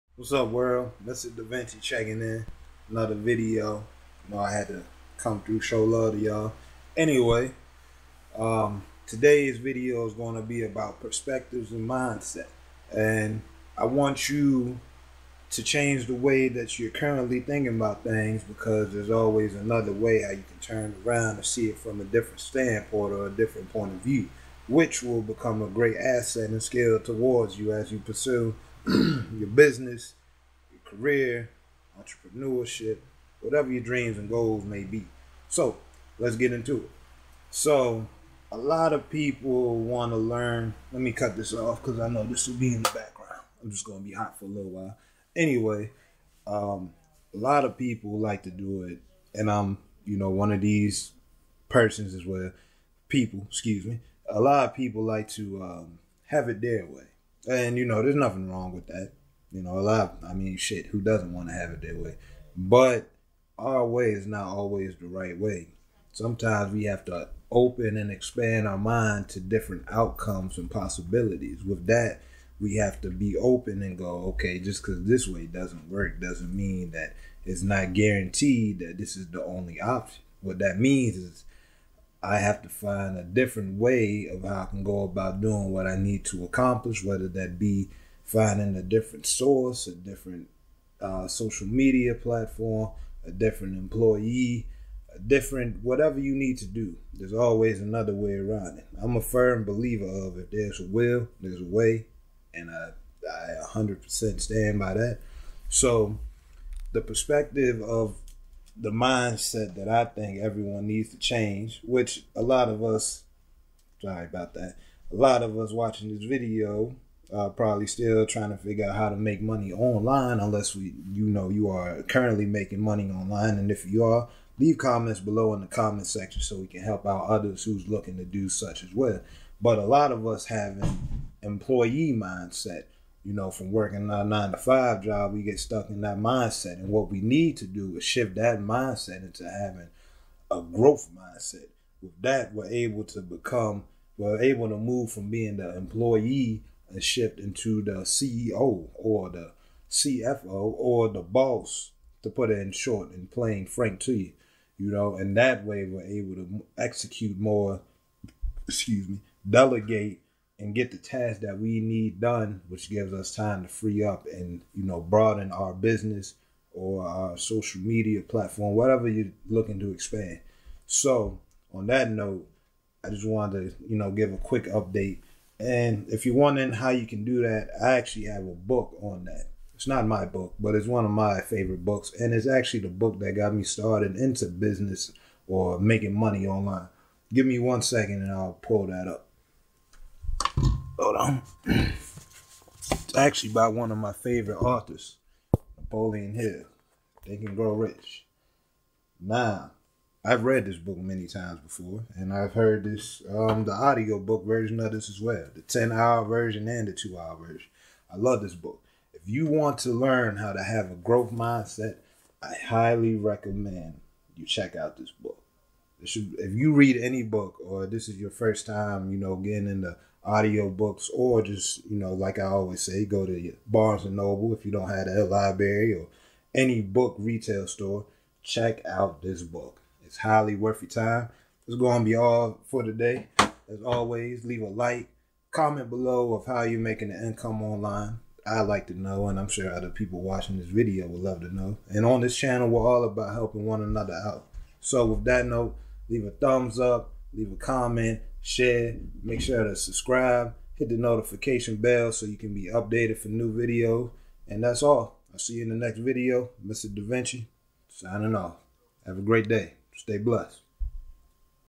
How To Develop A Growth Mindset
Today's Video I wanted to discuss how to develop a growth mindset. In this video I explain how to go from a employee mindset to a CEO mindset.